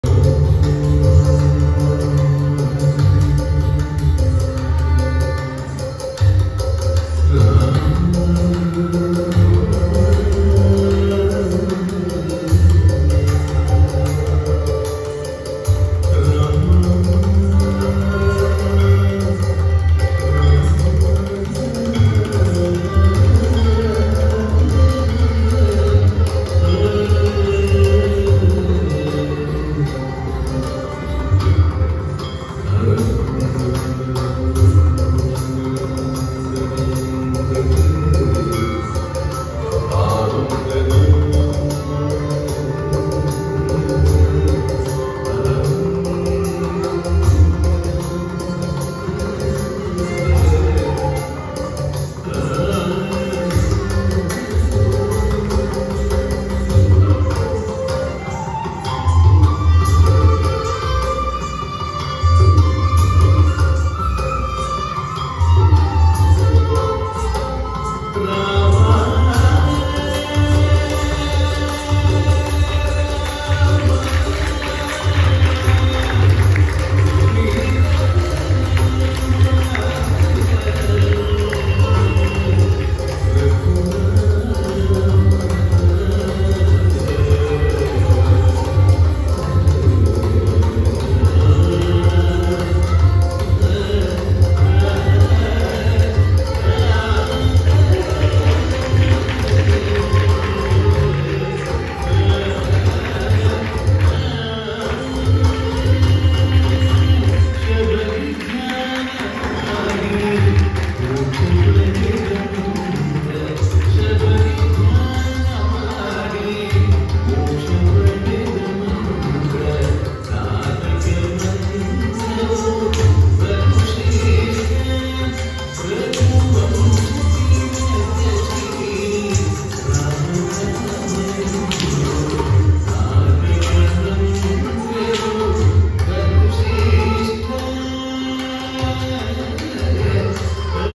SHABARI Moksha by Hema Prabhath
NRITHYA UTSAVA 2023  On AUG 8 th & 9 th 2023  Curated by @prashanthshastry @naatyakalakshetra45
Sukruthi Natyalaya presented
SHABARI MOKSHA

music by Vidwan Gurumurthy n Singer Vidwan Balasubramanya sharma CHOREOGRAPHED By hemaprabhath
Artist